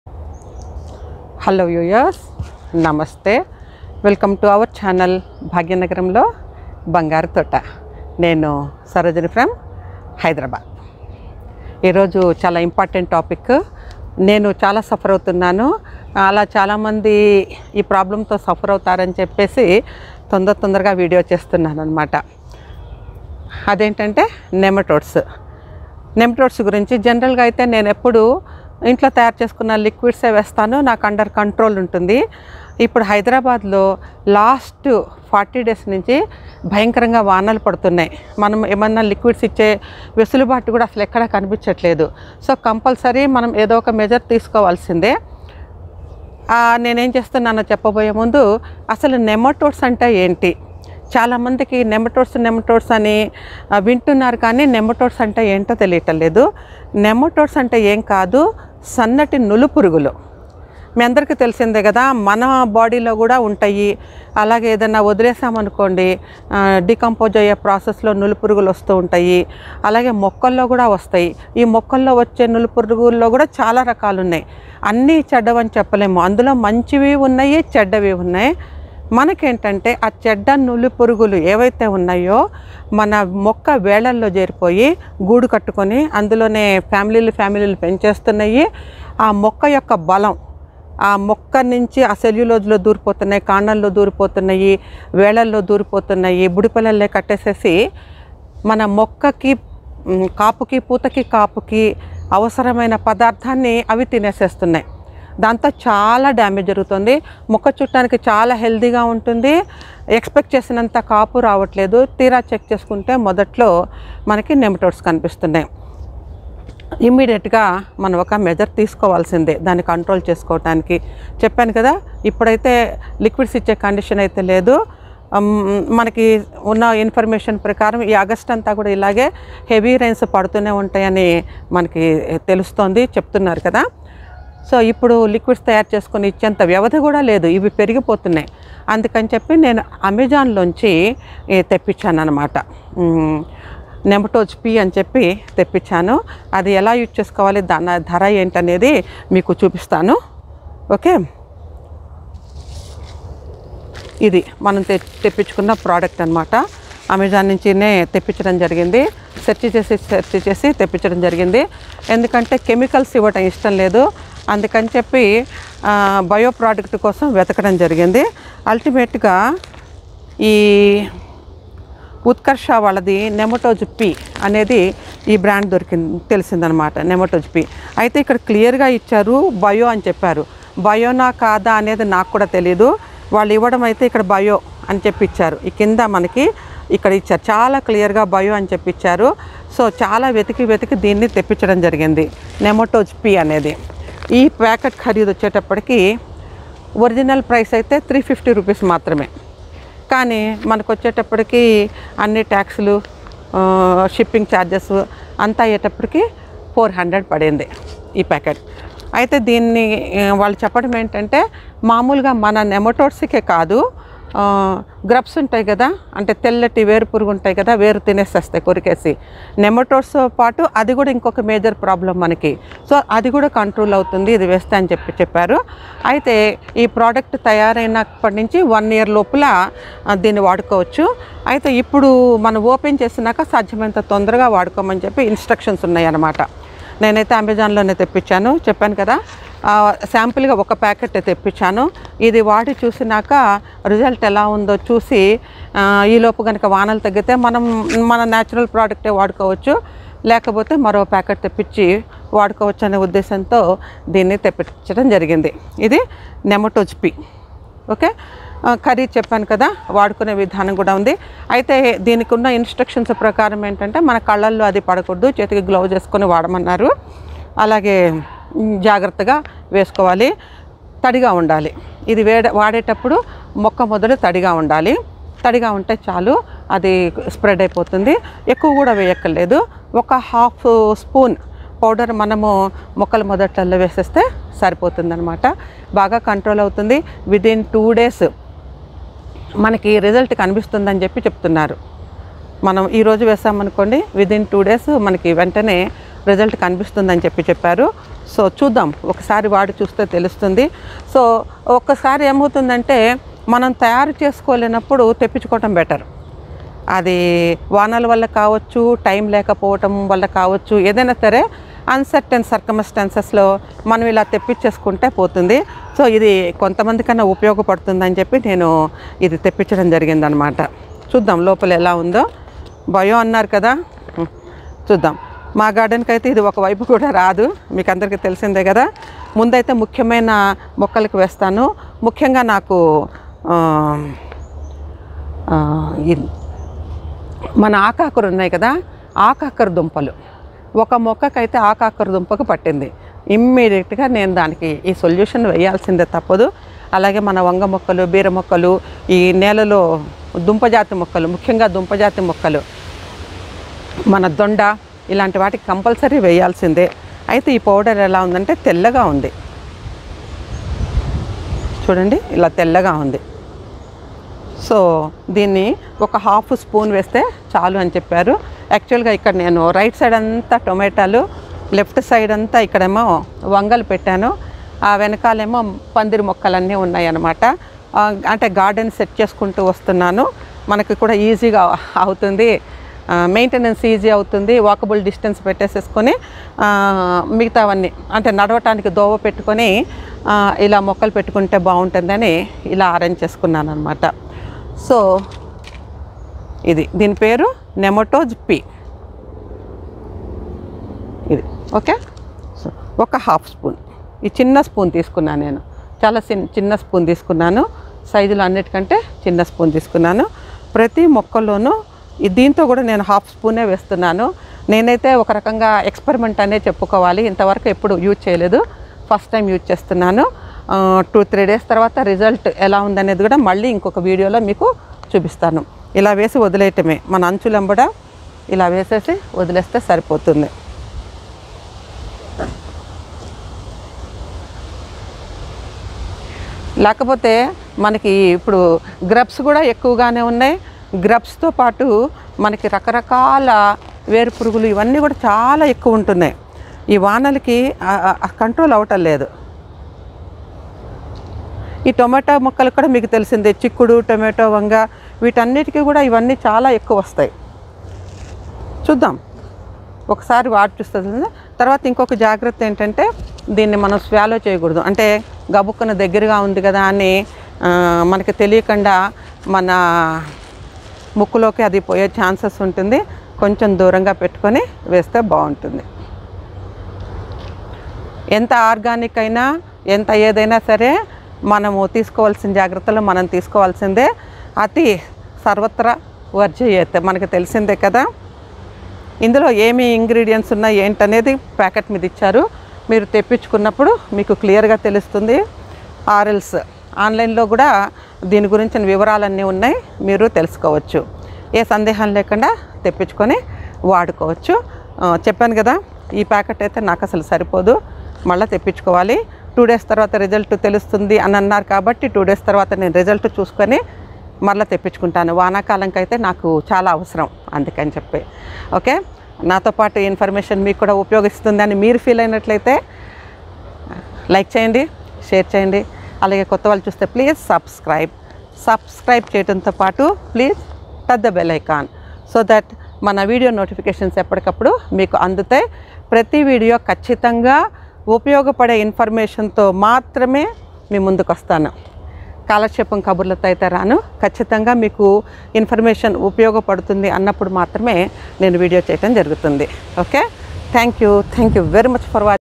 Single solution for nematodes and grubs which weaken plant roots.
This video talks about using a powder to reduce the effect of nematodes and grubs on plants.